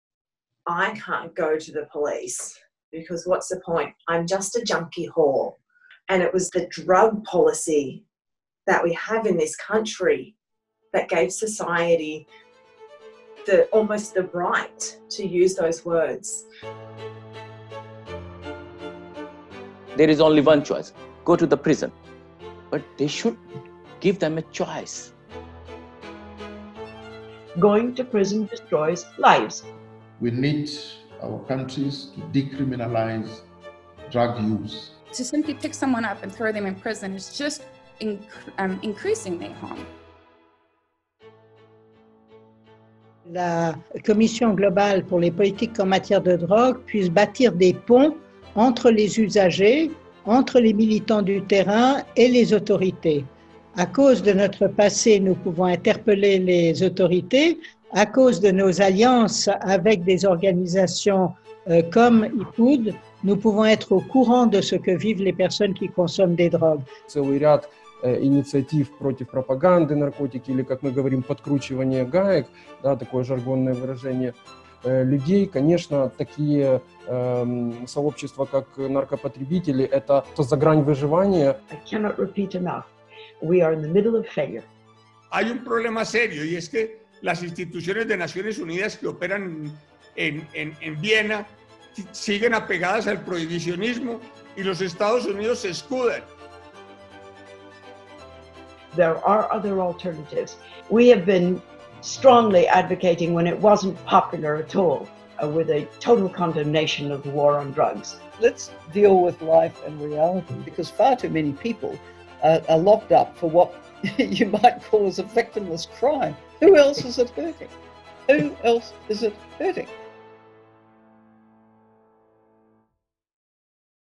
TRAILER: 'COVID-19 and People Who Use Drugs' Dialogue Series
Watch the trailer for our 9 part dialogue series, featuring members of the Global Commission on Drug Policy talking to people from the International Network of People Who Use Drugs and other global activist organisations.